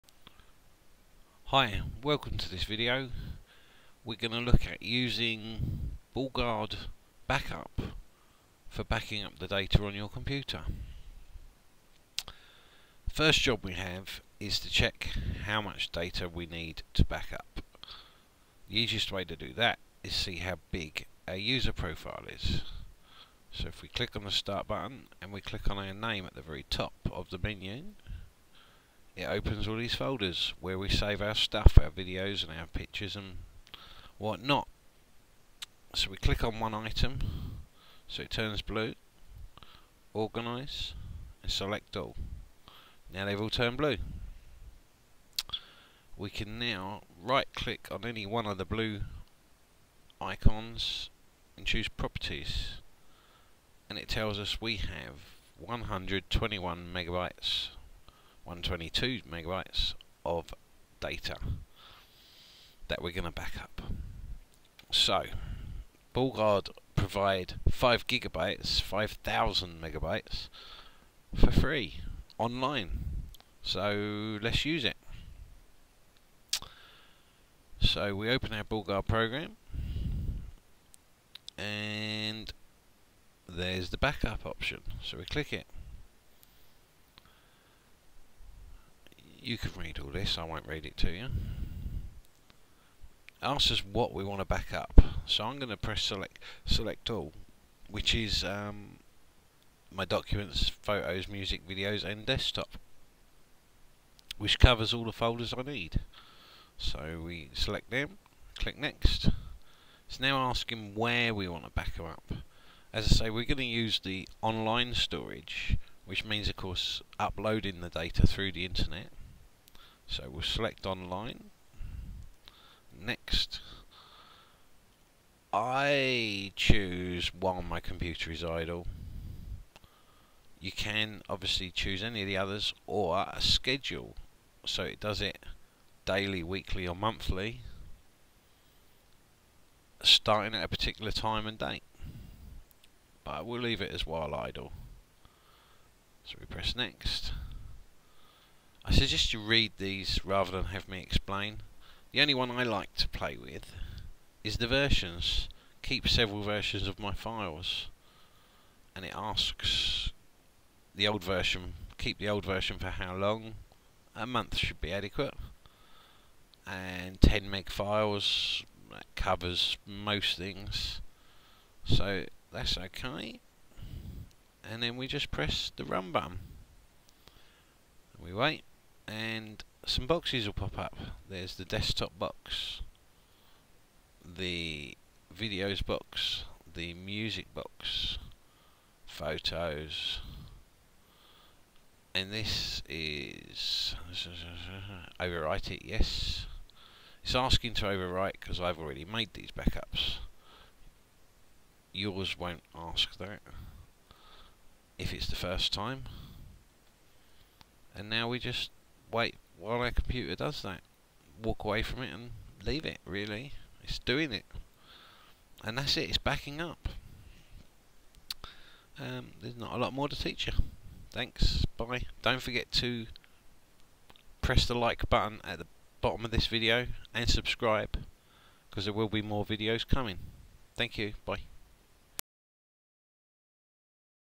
Using Bullguard Internet Security Or Bullguard Backup
A video explaining how to backup your Windows 7 pc data with Bullguard Internet Security Or Bullguard Backup